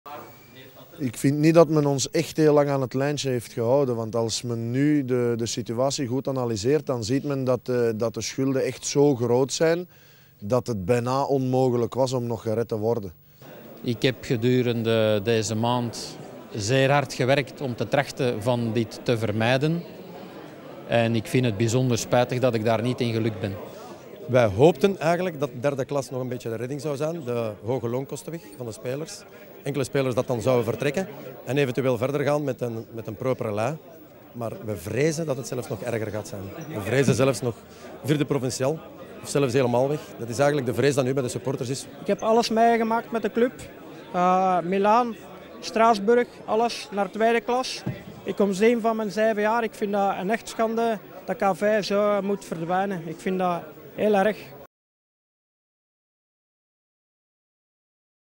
Throwback 2002: 20 jaar geleden ging KV Mechelen in vereffening
20 jaar geleden, op 7 december 2002, leek het einde nabij voor KV Mechelen. Na het vertrek van de flamboyante voorzitter Willy Van den Wijngaert nam een crisisbestuur met o.a. Johan Willemen de leiding van de club over. Maar dat bestuur moest al snel de handdoek gooien. De club ging wegens teveel schulden in vereffening. Voor de duizenden KV-fans stortte de wereld in. uitvoerig terug op de vereffening en de redding van Malinwa.